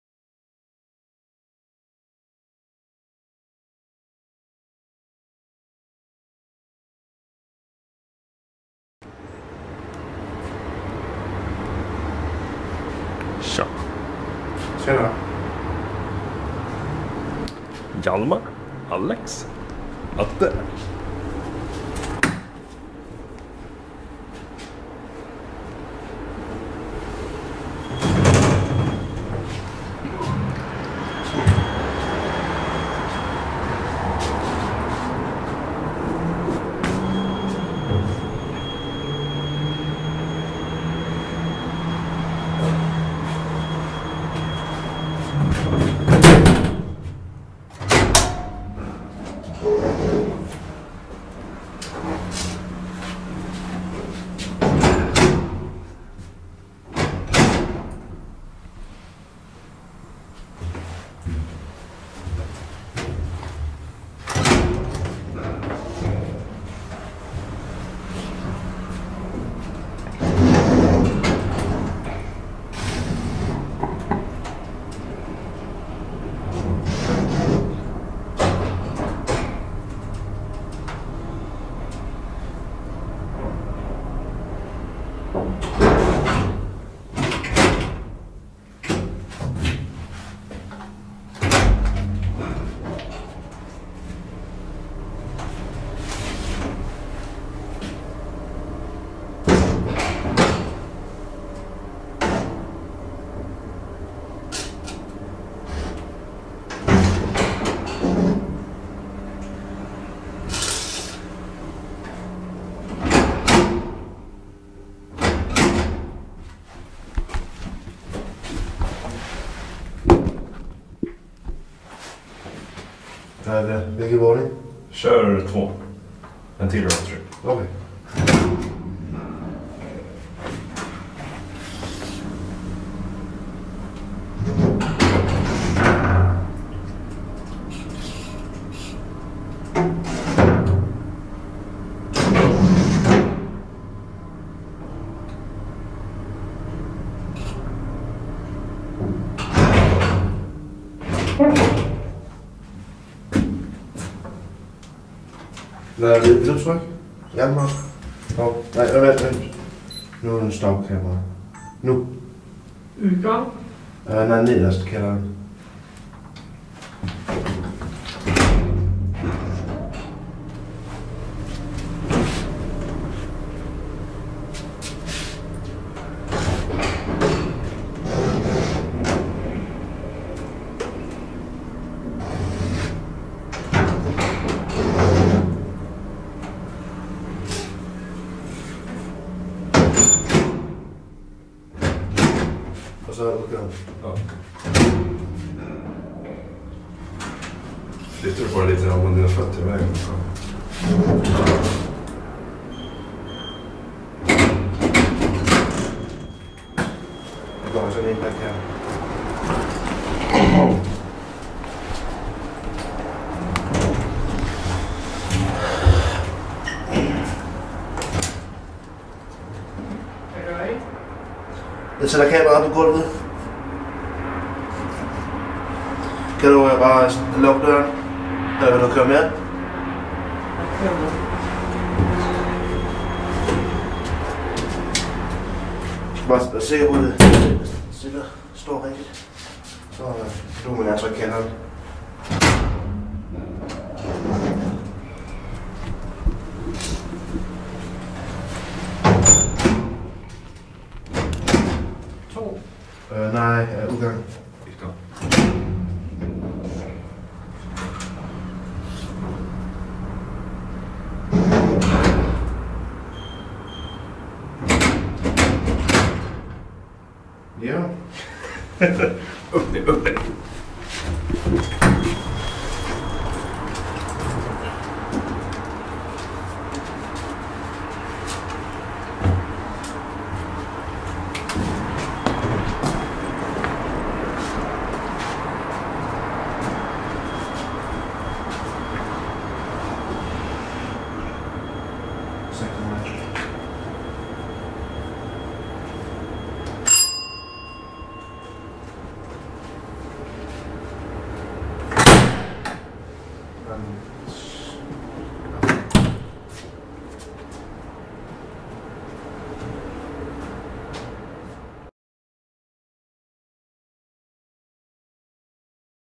TRIO TIME! Graham Brothers Traction Freight Elevators @ Ulvsundavägen 176A, Stockholm
Trio with @HeritageElevators and @hj43falk56

Manufacturer: Graham Brothers
Serial Number: 8011
Capacity: 1500Kg (_lbs.) - 15 Persons
Floors: 4 = K B 1 2

Location: Ulvsundavägen 176A, Stockholm (Ulvsunda) SWEDEN!